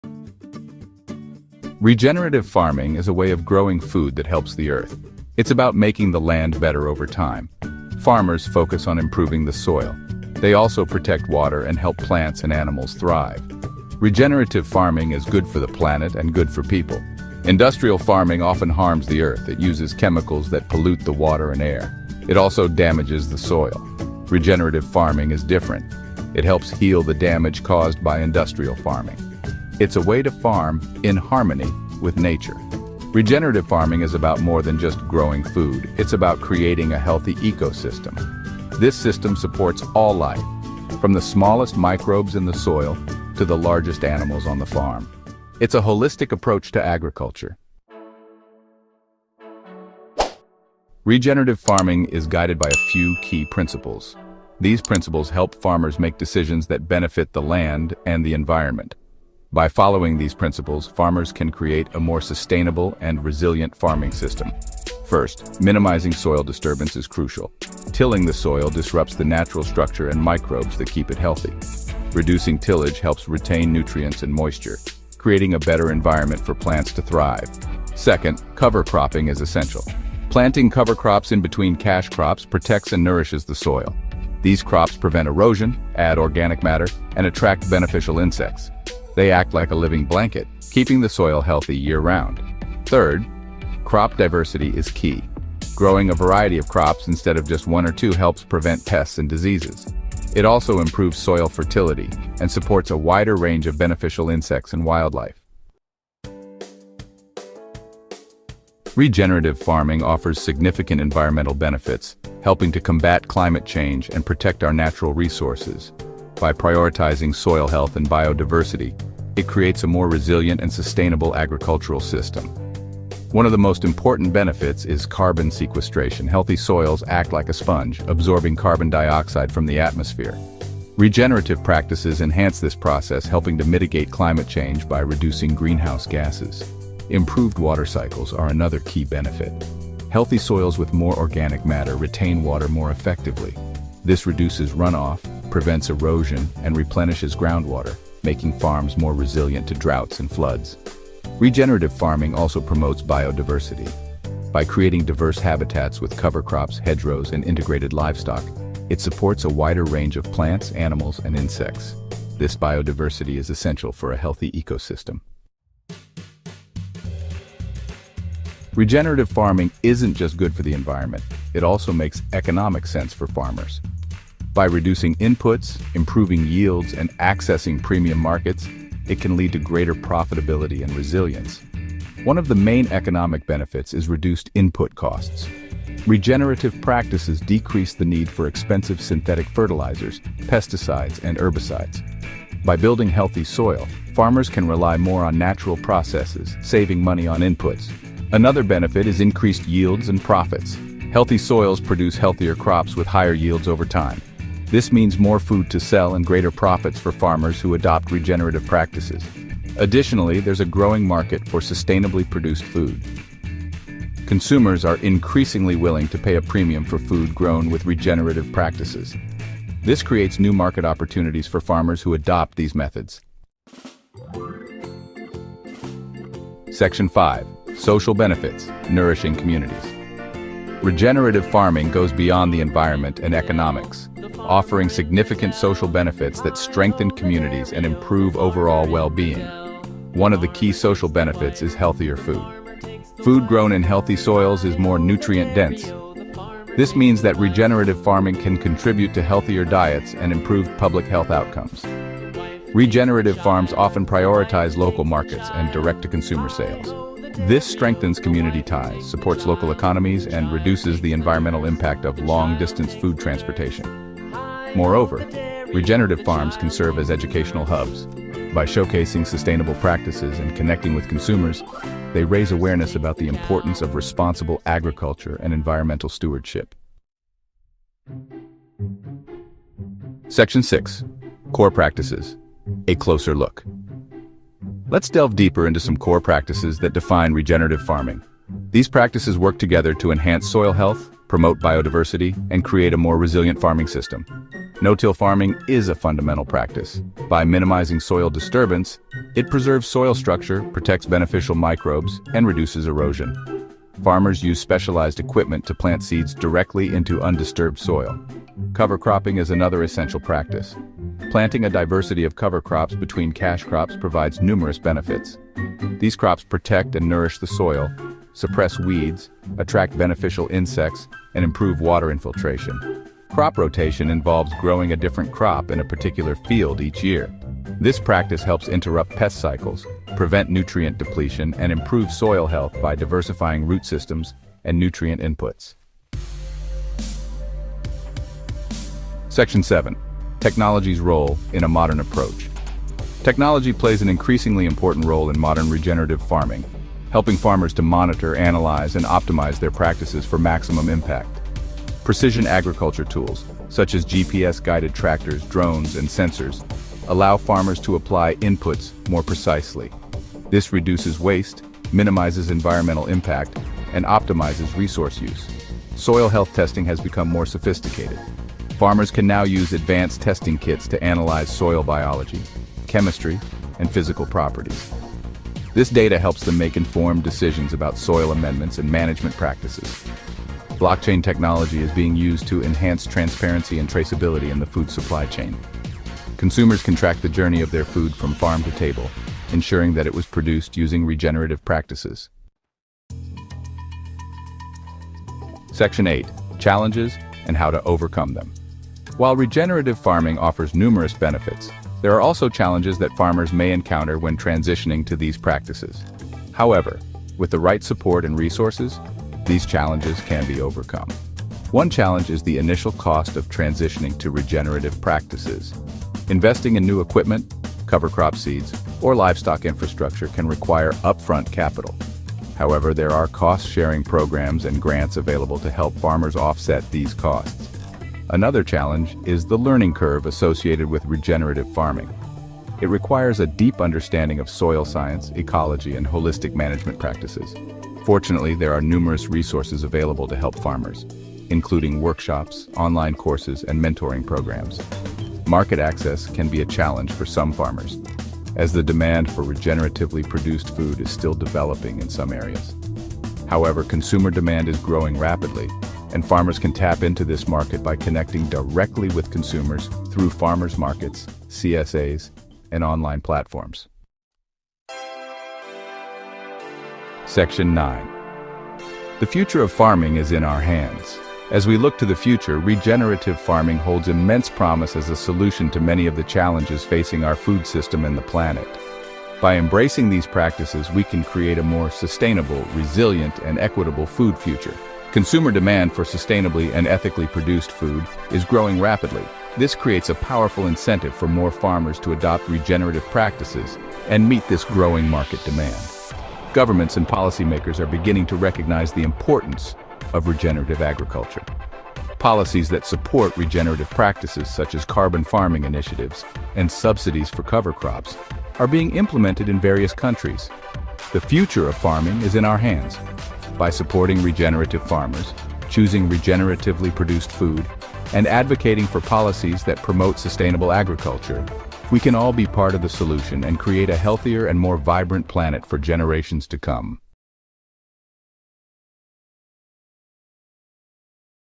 Everything You Need to Know About Regenerative Farming
Discover how regenerative farming is transforming agriculture! In this video, we cover everything you need to know — from soil health and carbon capture to real-world examples of regenerative practices. Whether you're a farmer, student, or eco-conscious consumer, this guide breaks it down simply and effectively.